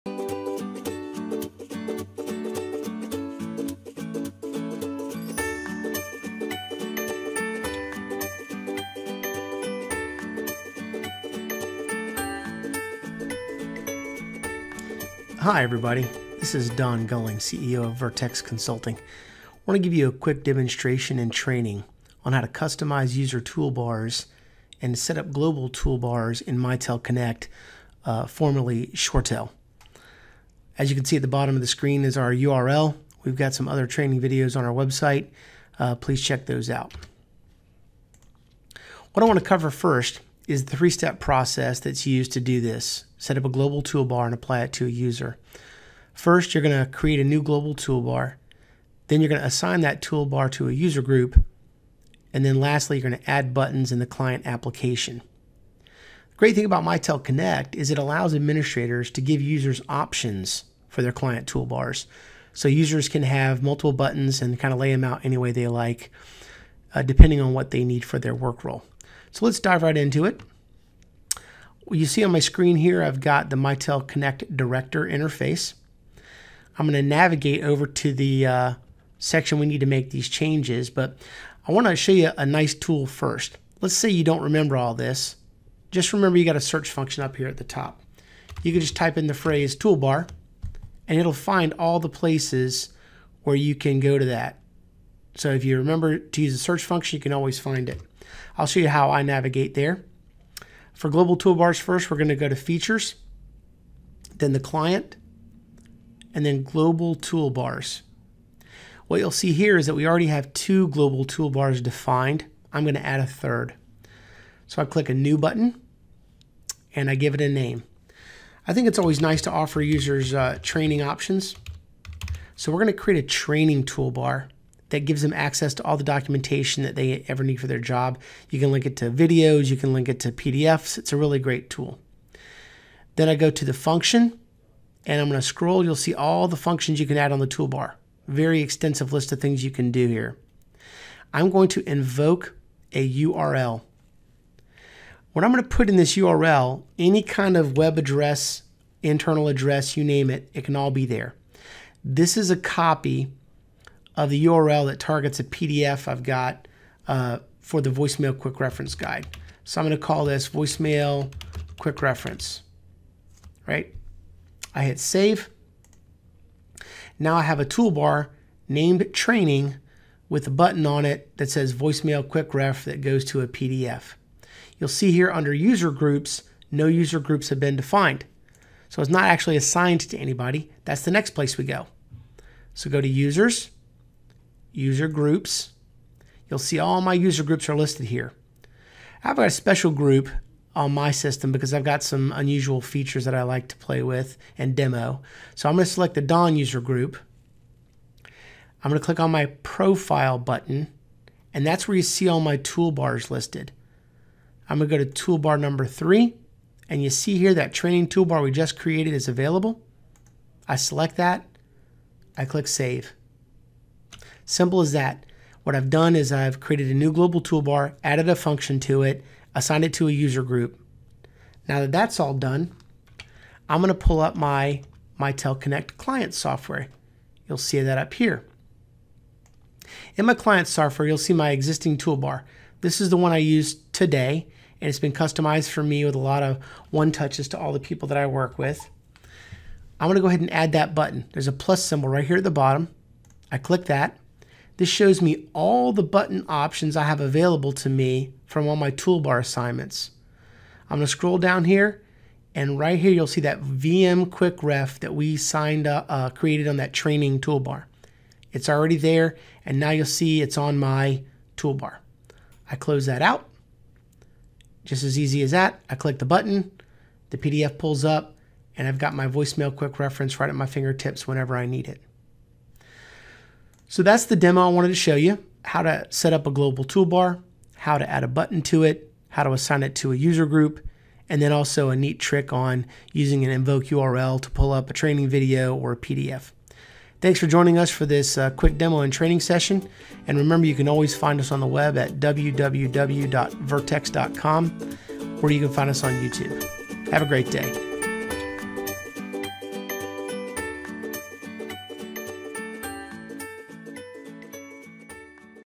Mitel Connect Training - How To Setup A Global Toolbar
This video shows you how to setup a global custom toolbar for your Mitel Connect (formerly ShoreTel) users.  Video covers setup of a new global toolbar, add a custom button to the new toolbar, and how to assign the toolbar to a user group.

The same basic principles apply to ShoreTel systems, so this video would be useful for ShoreTel system administrators as well.